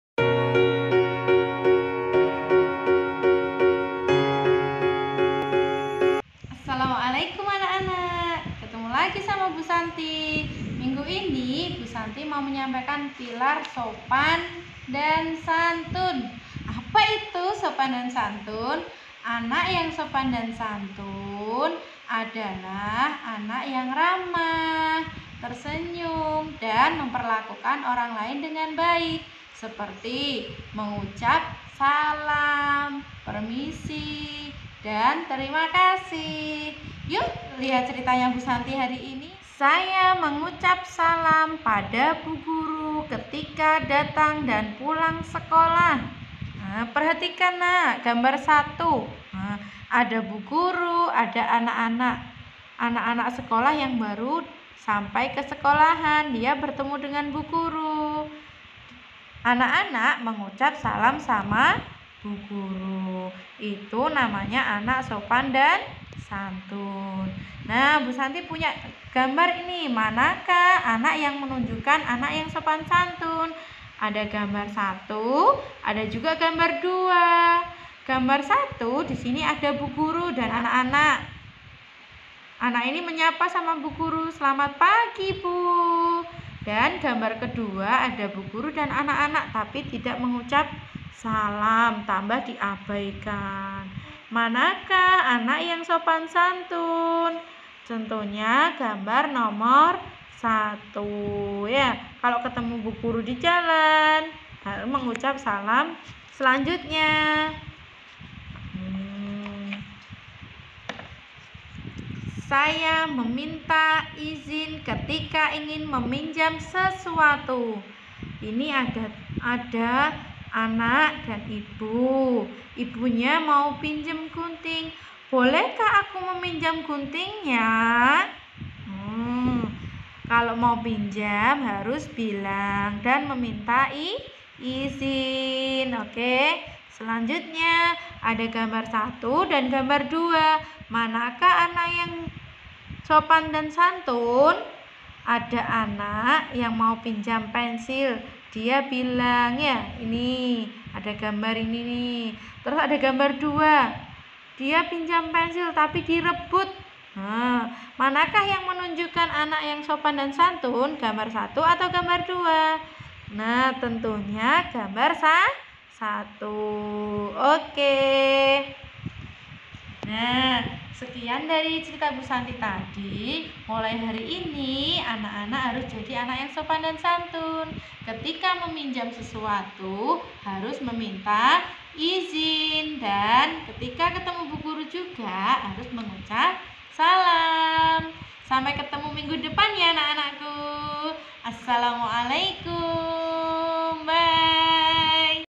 Konsep Pilar Karakter ~ Sopan dan Santun (Kelompok Usman)
Assalamualaikum Ayah Bunda dan Anak-Anak. Bagaimana kabarnya hari ini? Sudah siap belajar tentang sopan santun? Yukkkk kita simak videonya.